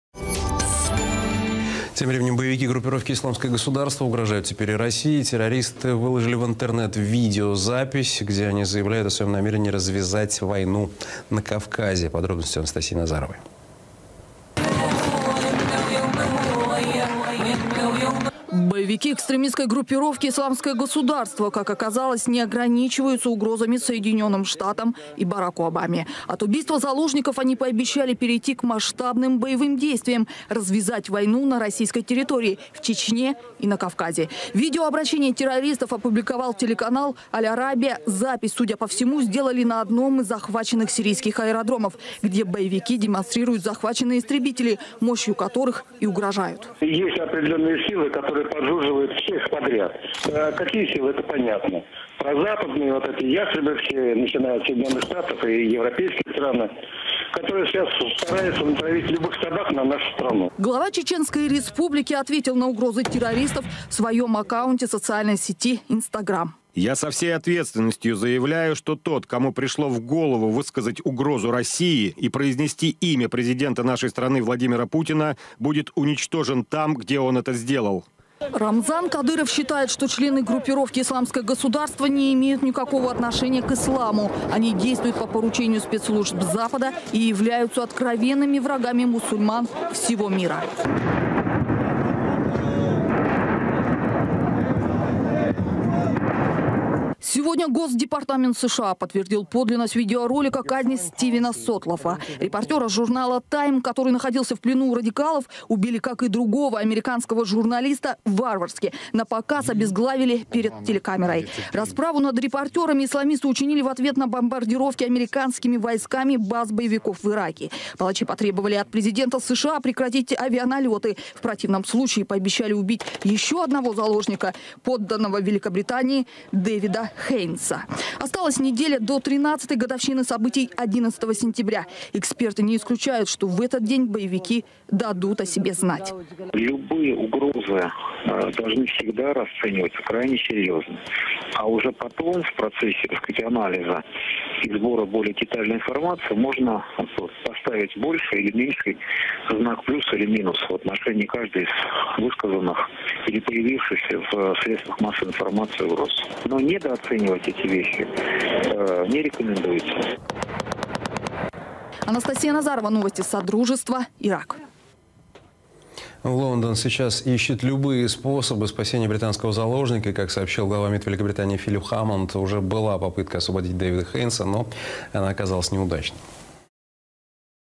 Путина оповестили, что Боевики ИГ вслед за США пригрозили России 2015
Путина оповестили, что Боевики ИГ вслед за США пригрозили России .
- наш канал Будьте в курсе самых актуальных новостей!